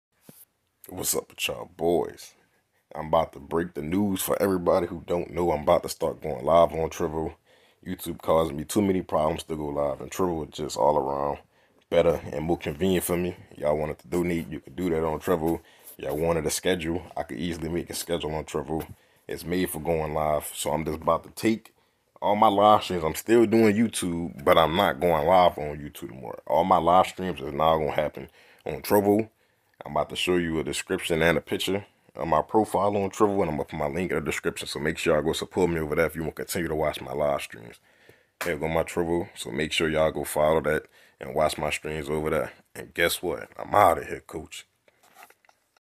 I'm Switching To Trovo
I'm switching to Trovo Live Streaming For Live Gameplays only I'm still doing YouTube 💪